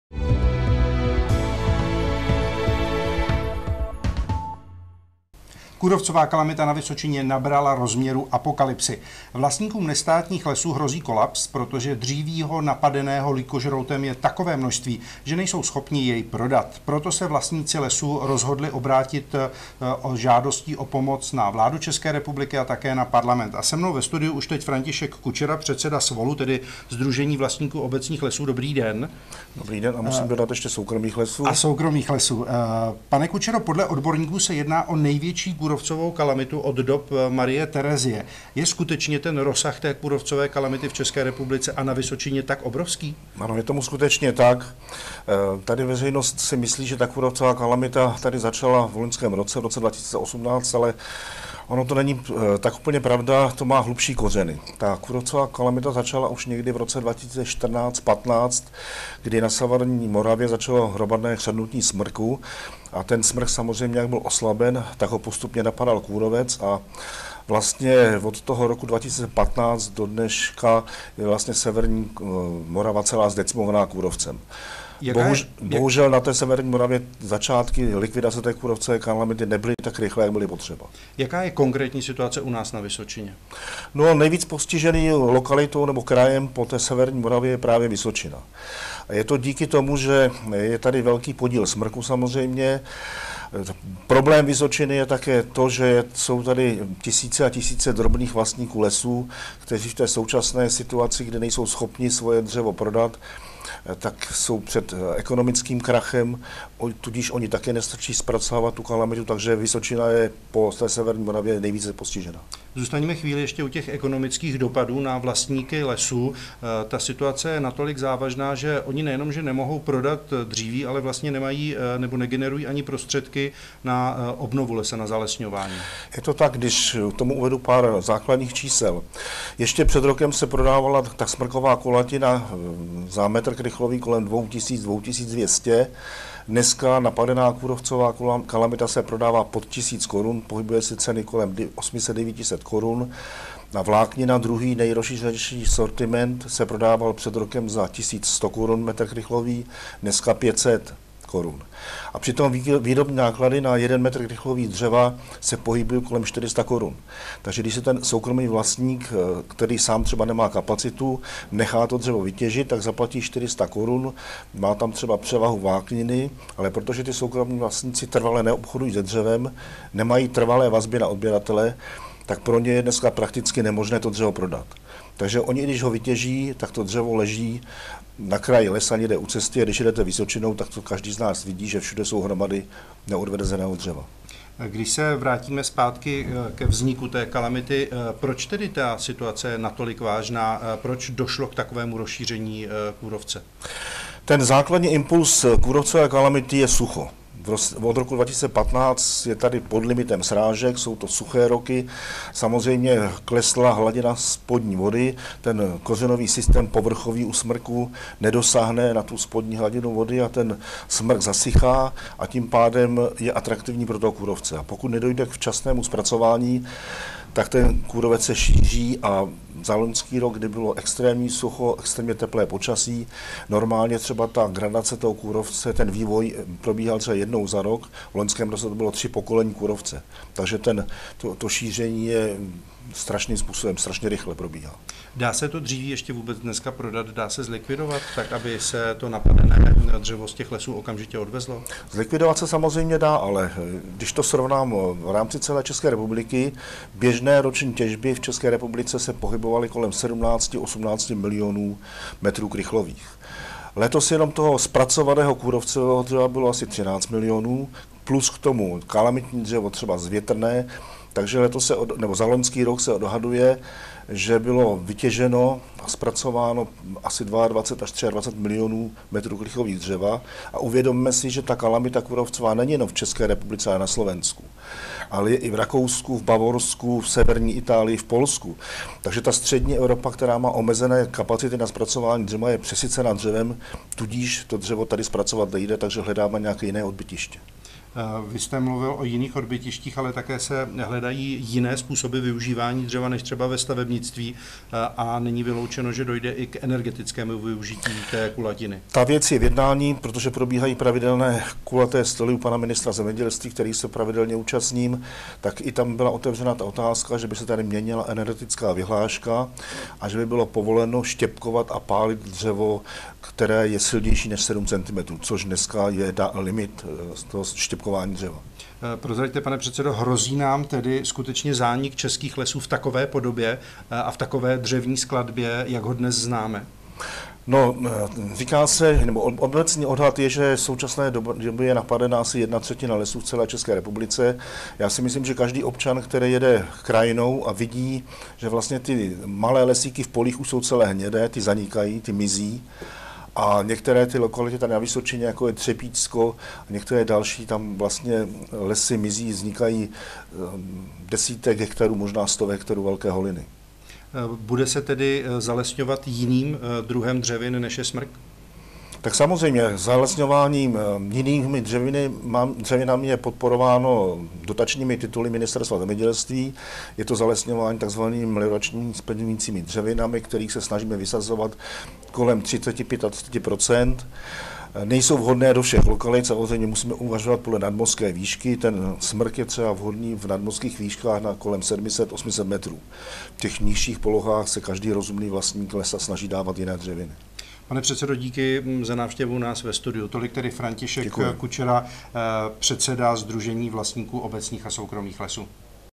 Kůrovcová kalamita nabrala rozměrů apokalypsy
Kůrovcová kalamita na Vysočině nabrala rozměrů apokalypsy. Vlastníkům nestátních lesů hrozí kolaps. Dříví napadeného lýkožroutem je tolik, že ho lesníci nestačí prodat. Proto se vlastníci lesů rozhodli sepsat petici a žádají vládu ČR a Parlament o pomoc. K nám do studia přišel o problematice promluvit František Kučera, předseda Sdružení vlastníků obecních a soukromých lesů.